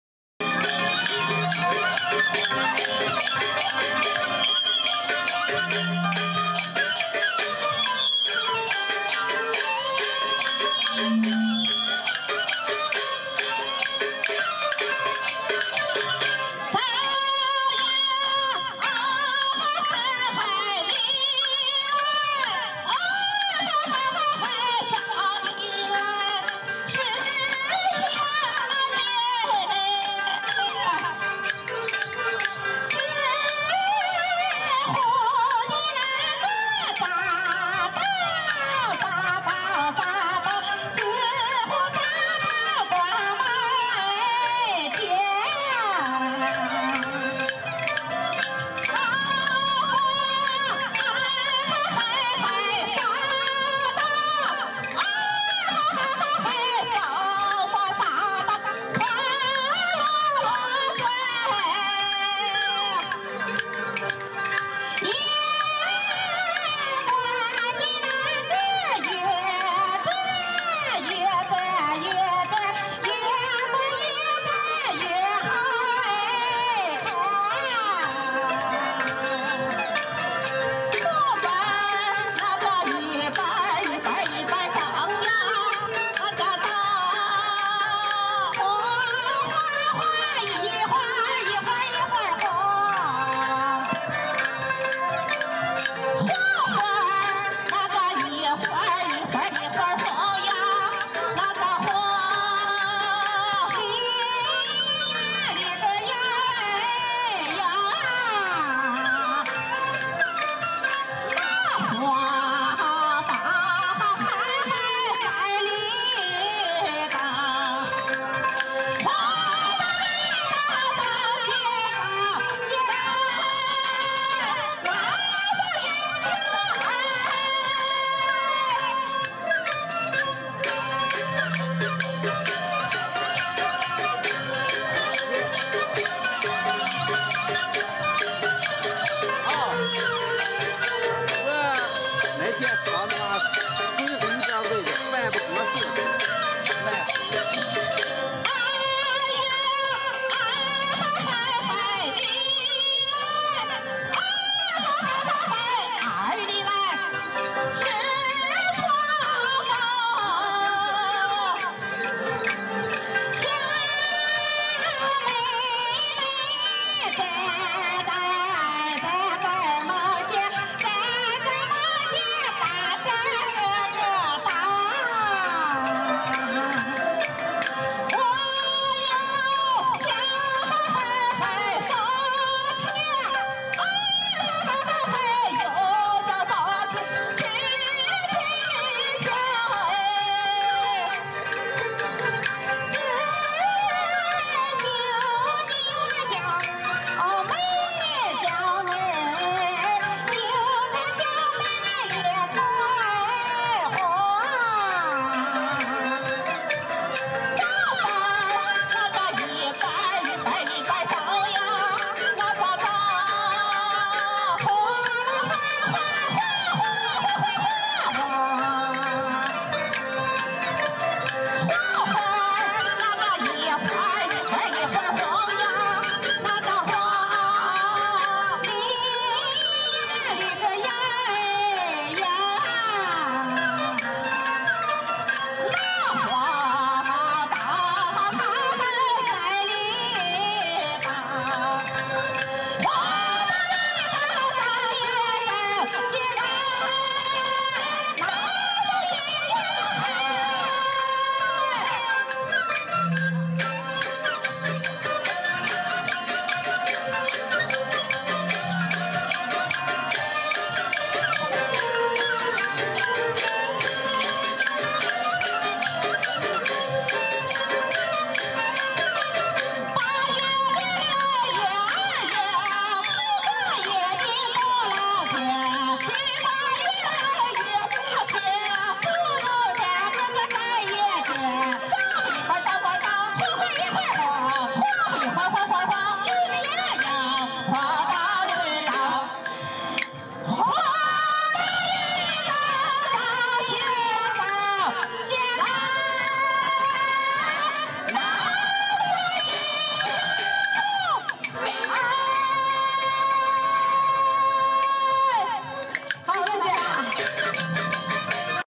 Zhangjiakou People's Park, two people, two people hanging red lights, Han Hua Zhang Li performa
Zhangjiakou People's Park, two people, two people hanging red lights, Han Hua Zhang Li performance